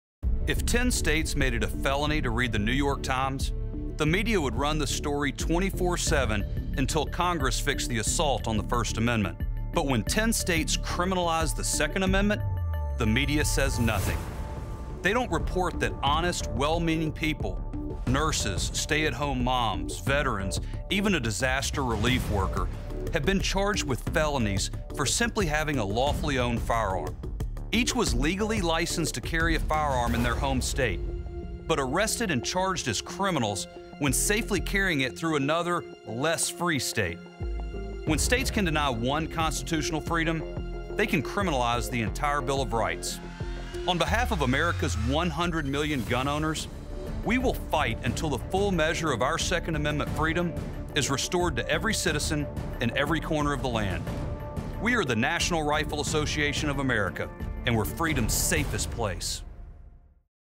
Freedom's Safest Place | Reciprocity
Chris W. Cox says states that deny one constitutional freedom criminalize the entire Bill of Rights. He promises to battle on behalf of America's 100 million gun owners until the full measure of our Second Amendment freedom is restored to every citizen in every corner of the land. The National Rifle Association fights for the protection of these liberties. The NRA is Freedom's Safest Place.